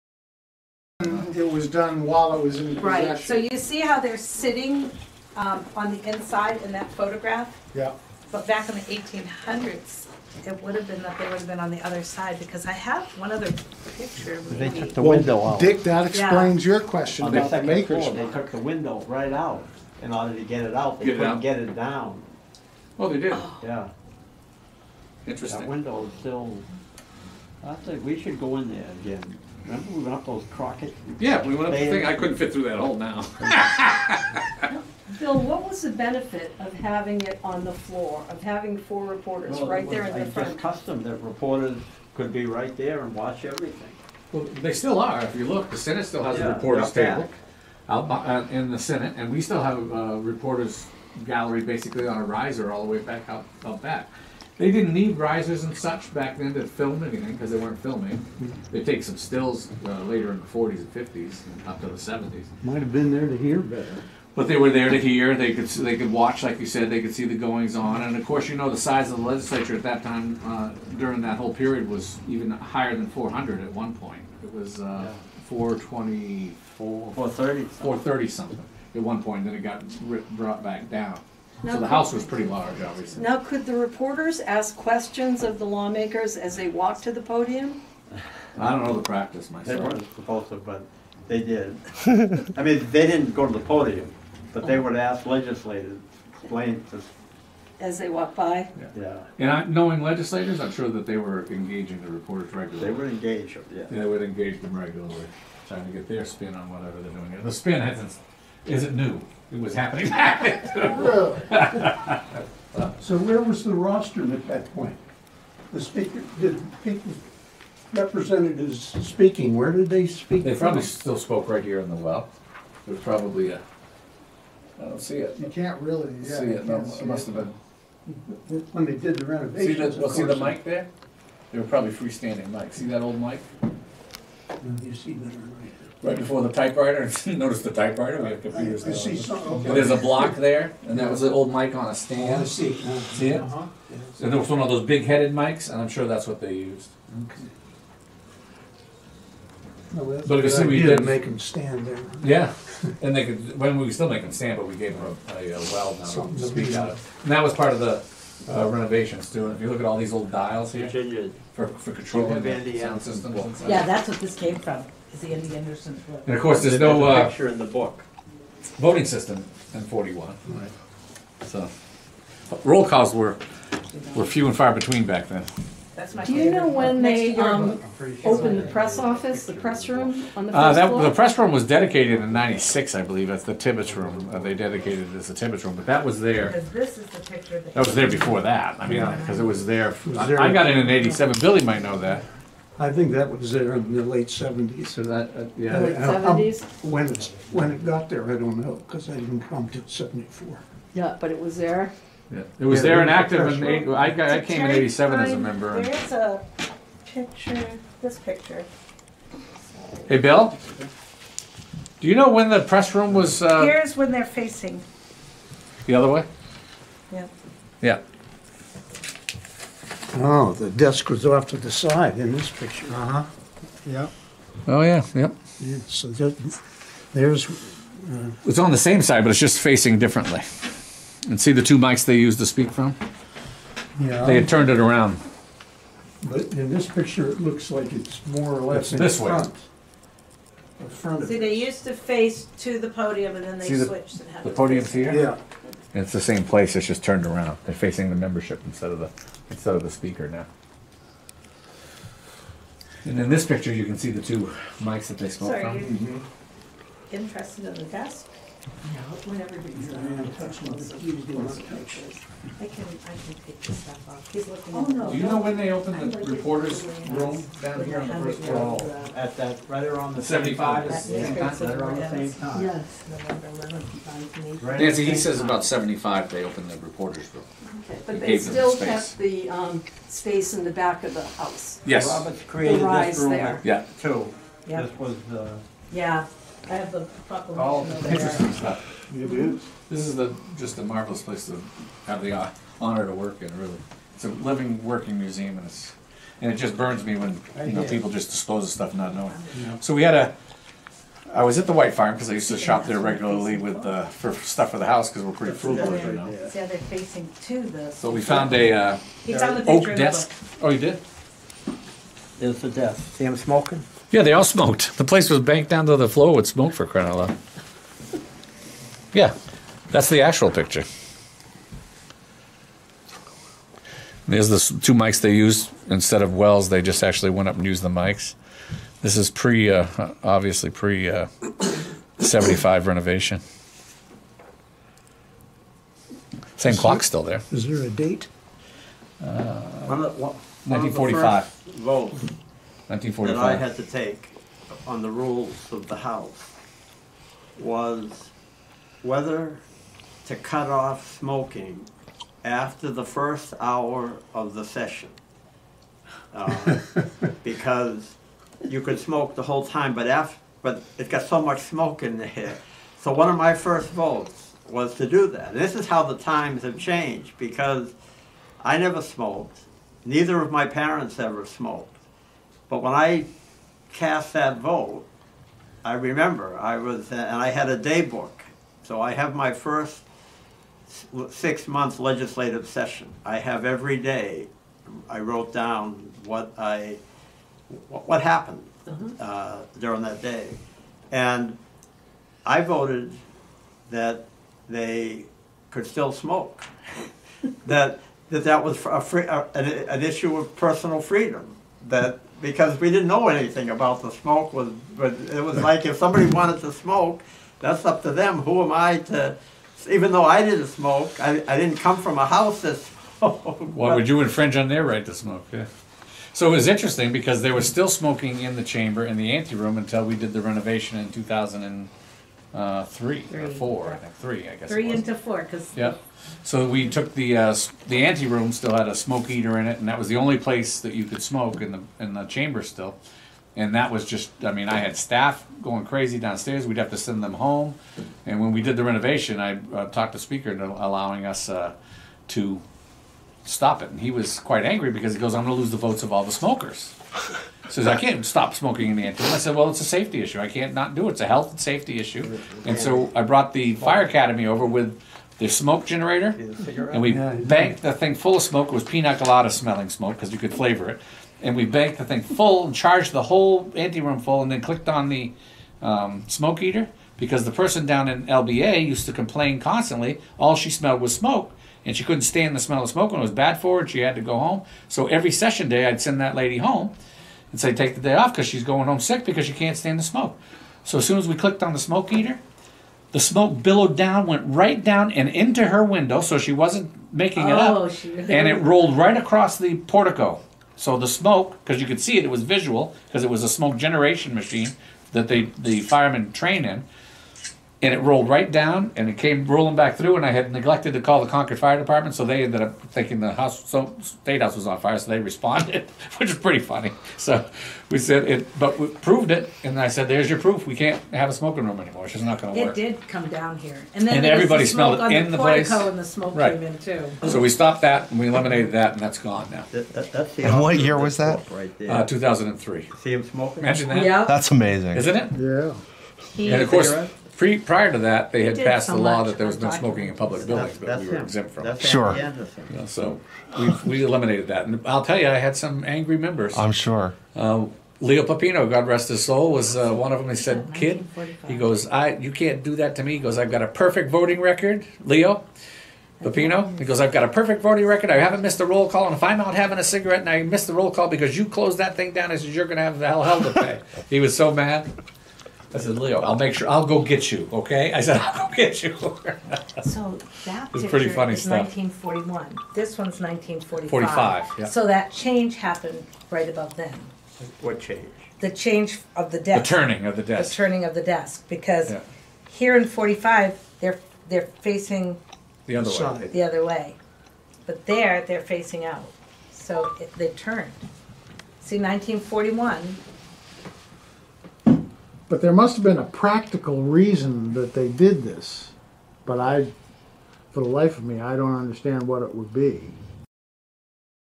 New Hampshire Bicentennial -- House Reporters Desk Mystery -- Part Three
(PART THREE OF THREE)

Terry Pfaff, chief of staff for the New Hampshire House of Representatives, joins Secretary of State Bill Gardner and Richard A. Hesse, University of New Hampshire professor emeritus, in exploring the history behind a once-sold -- and since reacquired -- antique desk used by reporters at the New Hampshire State House. (Video by Evan Lips for the New Hampshire Center for Public Interest Journalism -- for more,